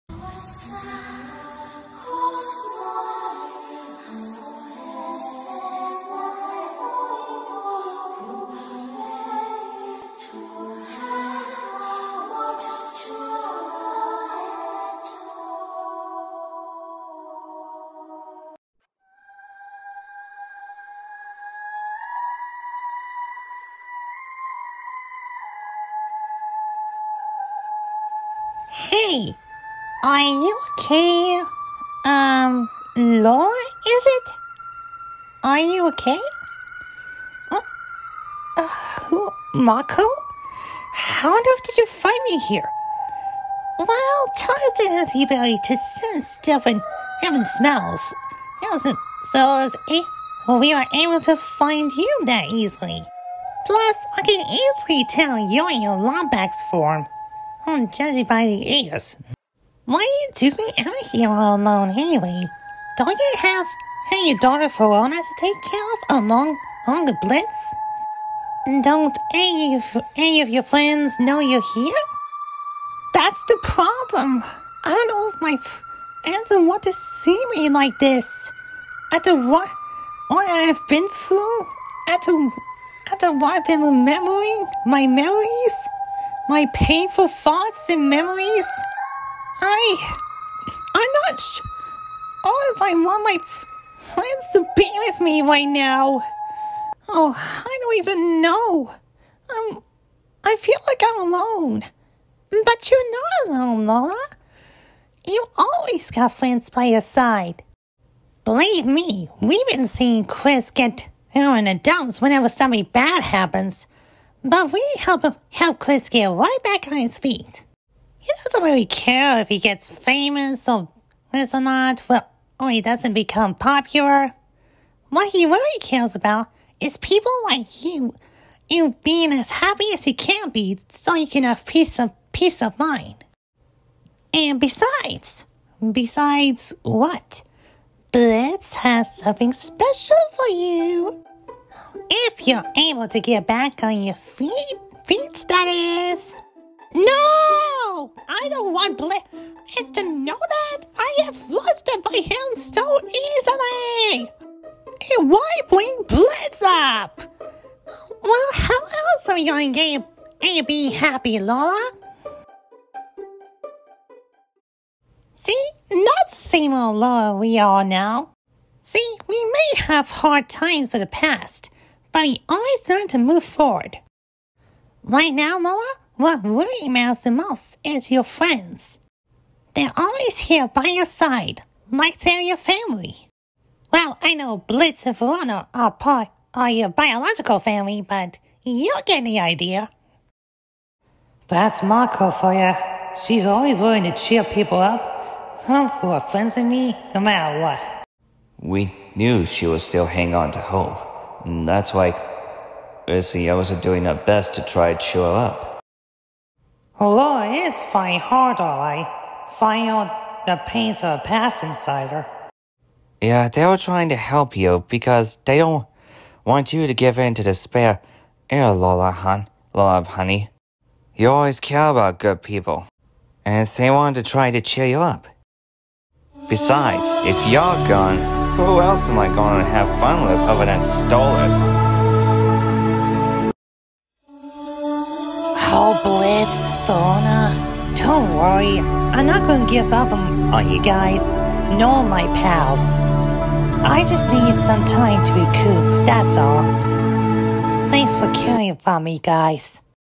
Mako has a talk with Laura
Laura (@Hiatusforawhile ) feels sad about remembering her painful past. But Mako Mankanshoku decided to have a talk with her about Friends, family, and compassion, hoping to cheer Laura up at least.

Age Requirement 13-16+ not for kids

Laura belongs to The Dark Crystal Empire (Empress Queen of darkness)